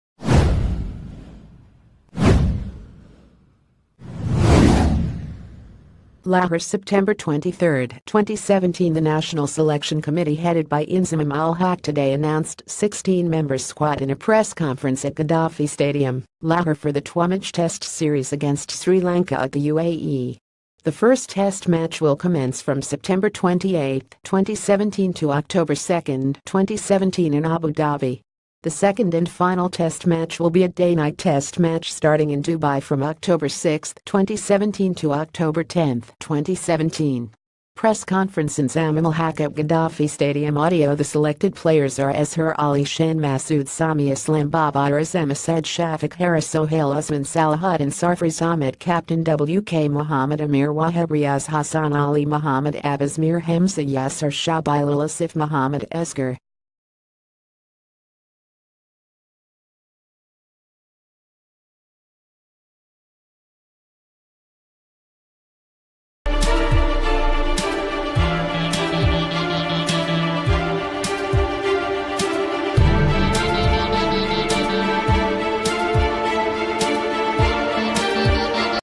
Breaking News | Pakistan test squad announced for the 2 test matches against sri lanka
Pakistan test squad announced for the 2 test matches against sri lanka

Lahore September 23, 2017 The National Selection Committee headed by Inzamam ul Haq today announced ...